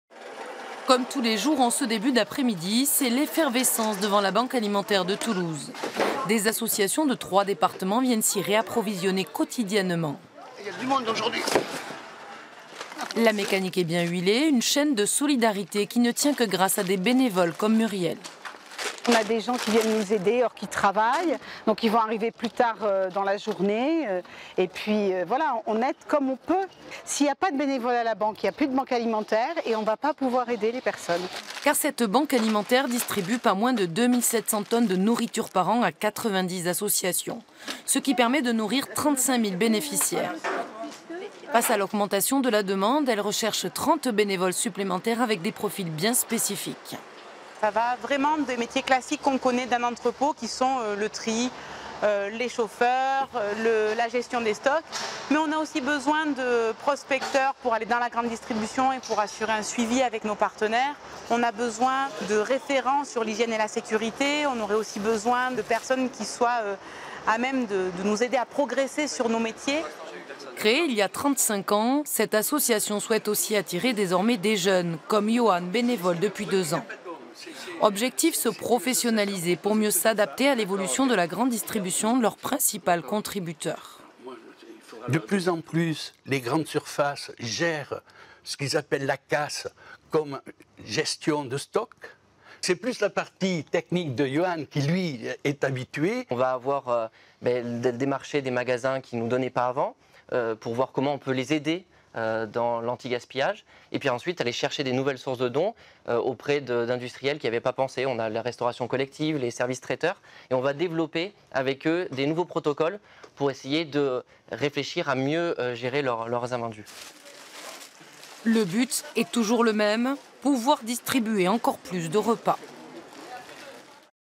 La Banque alimentaire de Toulouse cherche des bénévoles aux compétences multiples
La Banque alimentaire manque de bénévoles. Elle se restructure pour être plus performante et continuer de distribuer plus de 4 millions de repas aux plus démunis chaque année.